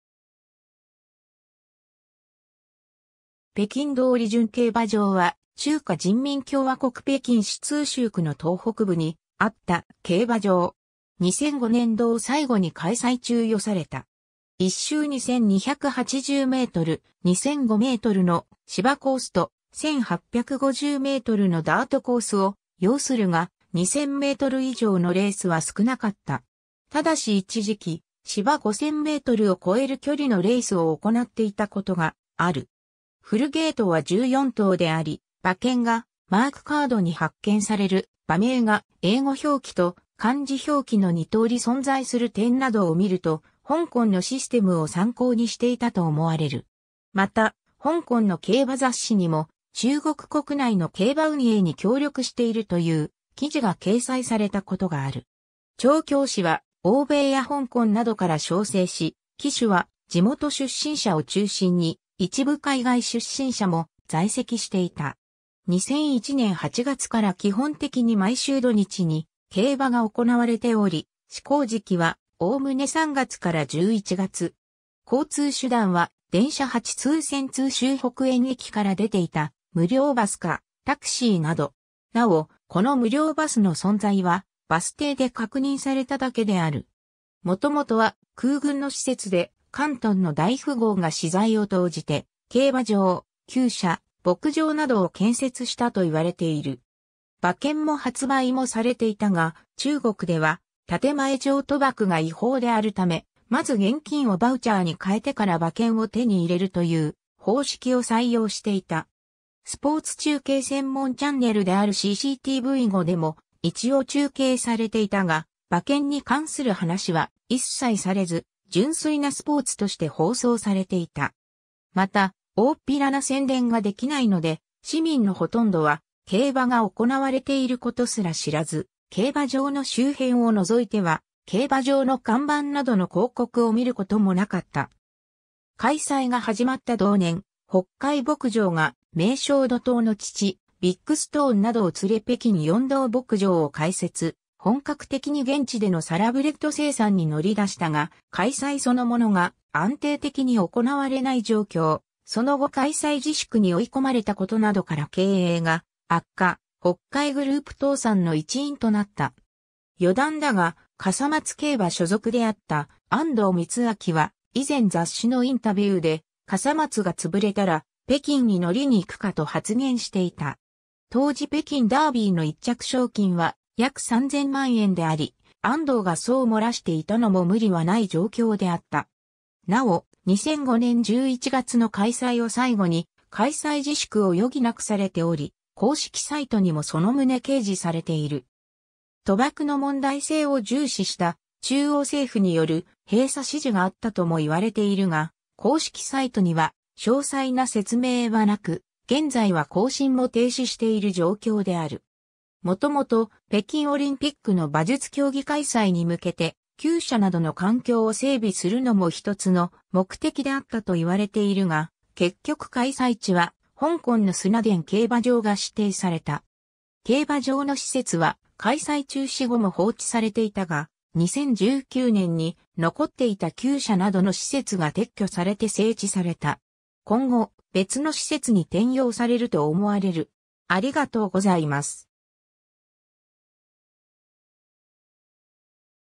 北京通順競馬場
北京通順競馬場（ペキンつうじゅんけいばじょう）は、中華人民共和国北京市通州区の東北部にあった競馬場。
2005年度を最後に開催中止された。
 一周2280メートル、2005メートルの芝コースと、1850メートルのダートコースを擁するが、2000メートル以上のレースは少なかった。
但し一時期、芝5000メートルを越える距離のレースを行っていた事がある。
フルゲートは14頭であり、馬券がマークカードに発券される、馬名が英語表記と漢字表記の2通り存在する点などを見ると、香港のシステムを参考にしていたと思われる。
また香港の競馬雑誌にも、中国国内の競馬運営に協力しているという記事が掲載された事がある。
 調教師は欧米や香港などから招請し、騎手は地元出身者を中心に、一部海外出身者も在籍していた。
 2001年8月から基本的に毎週土日に競馬が行われており、施行時期はおおむね3月から11月。
交通手段は電車八通線通州北苑駅から出ていた無料バス（最終便は午後1時半）か、タクシーなど。
なお、この無料バスの存在はバス停で確認されただけである。
 元々は空軍の施設で、広東の大富豪が私財を投じて競馬場、厩舎、牧場などを建設したと言われている。
馬券も発売もされていたが、中国では建前上賭博が違法であるため、まず現金をバウチャーに換えてから馬券を手に入れるという方式を採用していた。
スポーツ中継専門チャンネルであるCCTV5でも一応中継されていたが、馬券に関する話は一切されず、純粋なスポーツとして放送されていた。
また、大っぴらな宣伝が出来ないので、市民のほとんどは競馬が行われている事すら知らず、競馬場の周辺を除いては、競馬場の看板などの広告を見ることもなかった。
 開催が始まった同年、北海牧場がメイショウドトウの父・ビッグストーンなどを連れ北京龍頭牧場を開設、本格的に現地でのサラブレッド生産に乗り出したが、開催そのものが安定的に行われない状況、その後開催自粛に追い込まれたことなどから経営が悪化、北海グループ倒産の一因となった。
 余談だが、笠松競馬所属であった安藤光彰は以前雑誌のインタビューで、「笠松が潰れたら北京に乗りに行くか」と発言していた。
当時北京ダービーの1着賞金は約3000万円であり、安藤がそう漏らしていたのも無理は無い状況であった。
賭博の問題性を重視した中央政府による閉鎖指示があったとも言われているが、公式サイトには詳細な説明は無く、現在は更新も停止している状況である。
元々、北京オリンピックの馬術競技開催に向けて、厩舎などの環境を整備するのも一つの目的であったといわれているが、結局開催地は香港の沙田競馬場が指定された。
 競馬場の施設は、開催中止後も放置されていたが、2019年に残っていた厩舎などの施設が撤去されて整地された。
今後、別の施設に転用されると思われる。